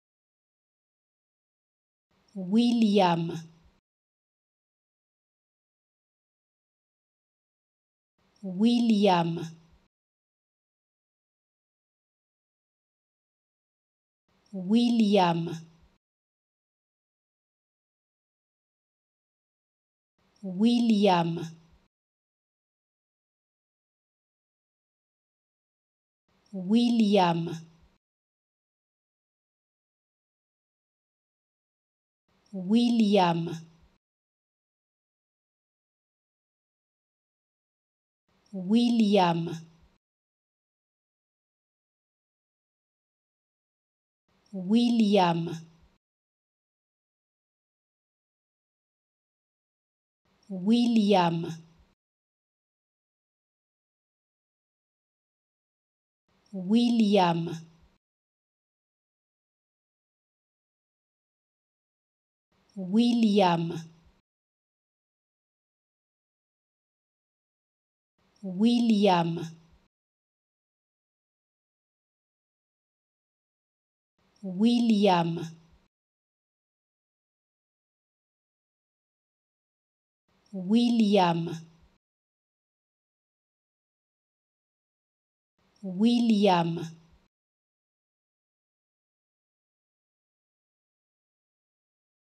How to Pronounce ''William'' Correctly in French
Learn how to say and properly pronounce ''William'' in French with this free pronunciation tutorial. Apprenez à prononcer des mots en français grâce à nos tutoriels.